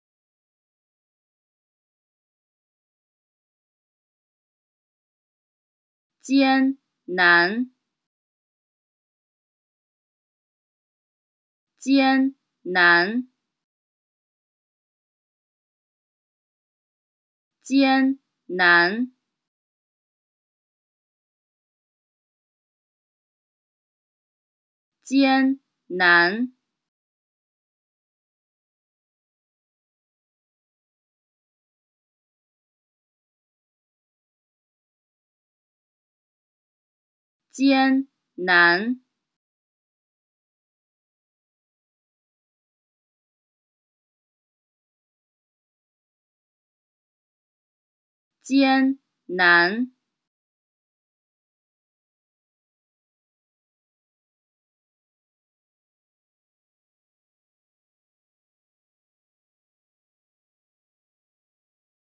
How To Say ''Hard'' (艰难) in Mandarin Chinese
Learn how to pronounce ''Hard To Avoid'' (难免) in Mandarin Chinese with this short pronunciation video